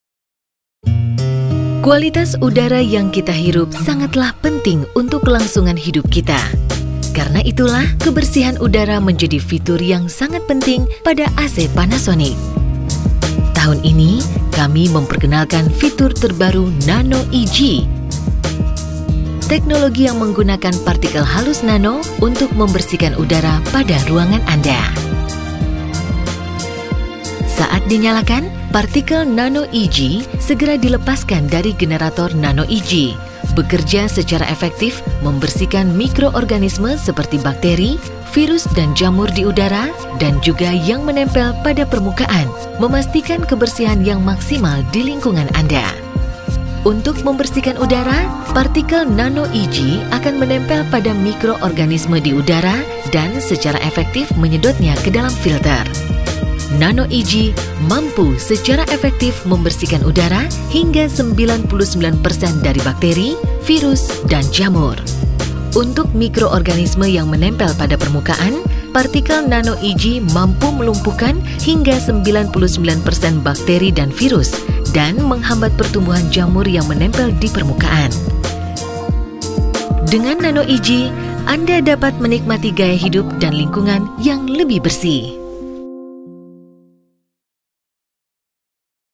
Nanoe-G - Panasonic Air Conditioner
New Panasonic Air Conditioner 2012 dengan feature terbaru NANOE-G.
NANOE-G dapat membunuh kuman dan virus tidak hanya yang ada di udara tapi juga yang ada di permukaan benda seperti dinding, kursi lantai dan lain sebagainya.